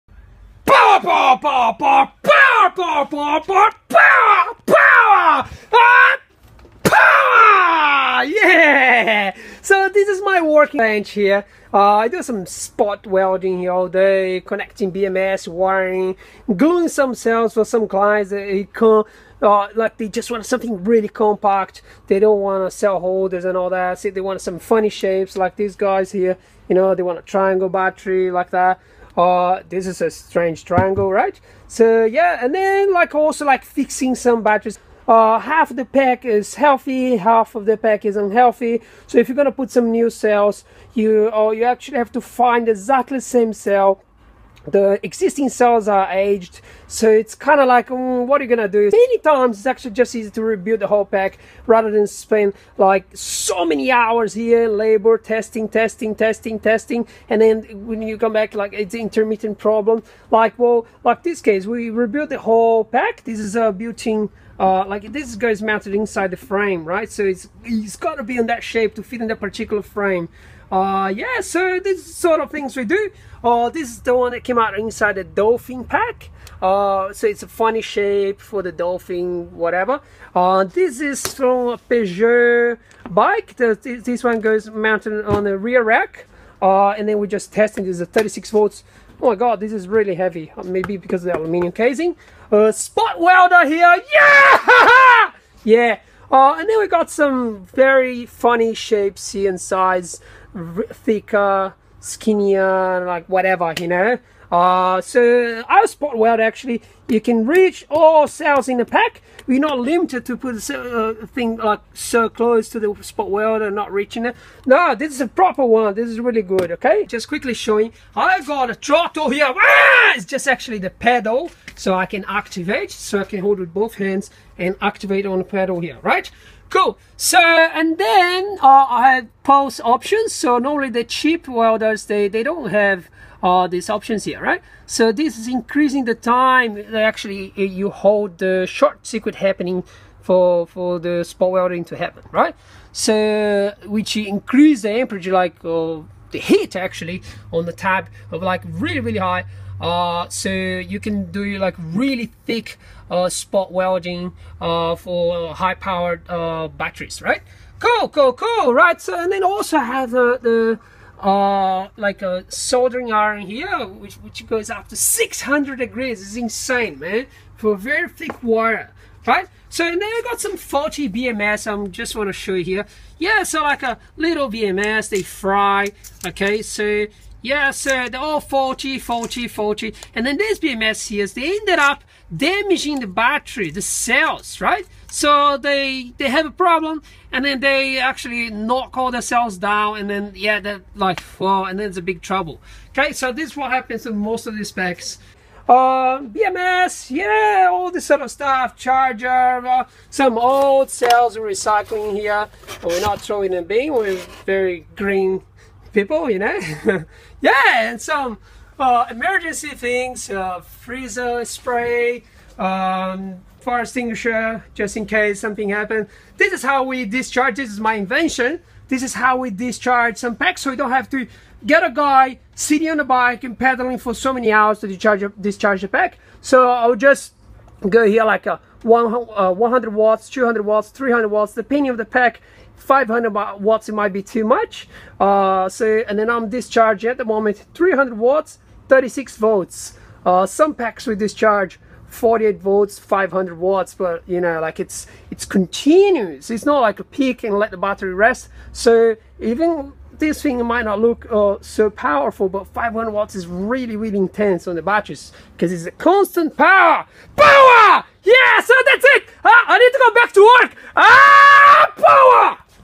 Battery work station: spot welder, custom packs, BMS • Tour at eMax-eBikes lab • Electric Bike
They also sell extremely powerful BMS (up to 150A), they have:
14s 52v = you have to email them and request it

14s 30A BMS =
14s 150A BMS =
10s 36v BMS =

Have fun shopping... don't forget the FIRE EXTINGUISHER and protective glasses!